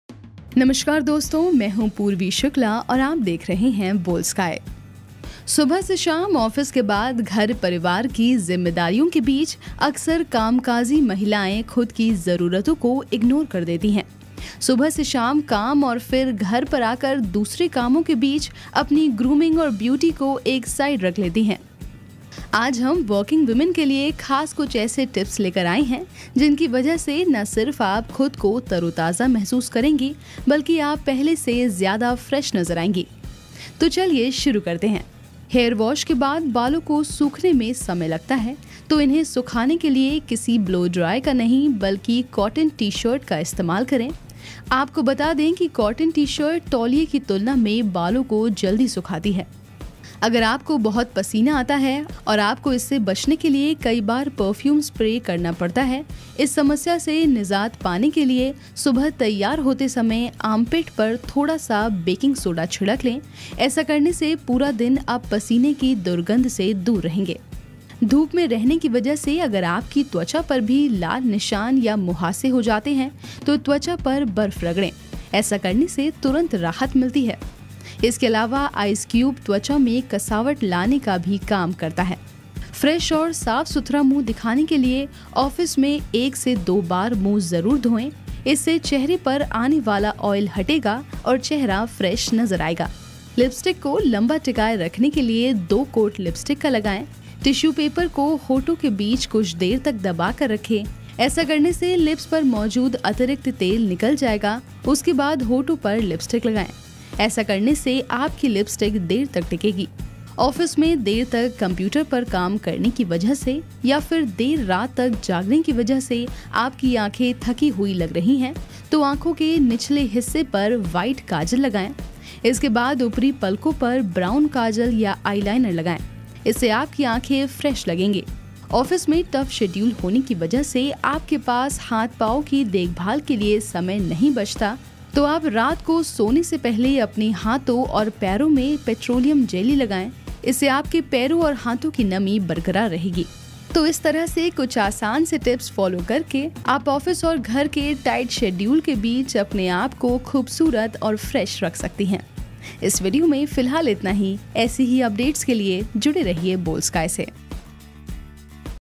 Tips to look Fresh in Office | ऑफिस में दिखना चाहतीं हैं फ्रेश, तो जरूर अपनाएं ये नुस्खे | Boldsky
Keeping yourself fresh and clean in office becomes a difficult task on daily basis, espescially during summers. We often don’t understand how we can get rid of this situation. In this video we are telling you about some quick tips that you can follow to keep yourself fresh and clean throughout the day.

सुबह से शाम काम और फिर घर पर आकर दूसरे कामों के बीच अपनी ग्रूमिंग और ब्‍यूटी को एक साइड रख लेती है। आज हम वर्किंग वीमन के लिए खास कुछ ऐसे टिप्‍स लेकर आए है जिनकी वजह से न सिर्फ आप खुद को तरोताजा महसूस करेंगी बल्कि आप पहले से ज्‍यादा फ्रेश नजर आएंगी।